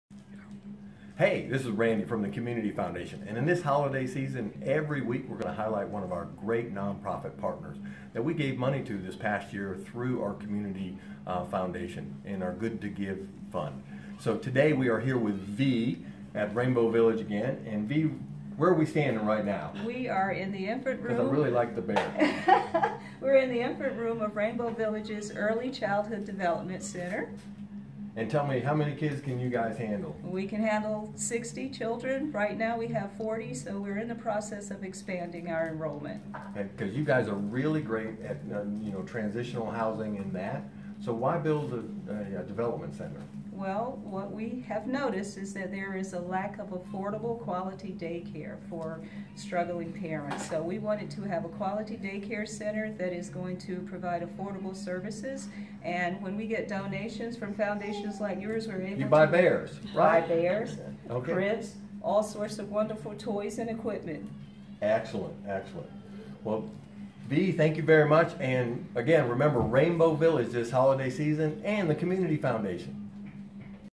Rainbow Village
See how Rainbow Village is using their 2017 grant money from our Good2Give Community Fund.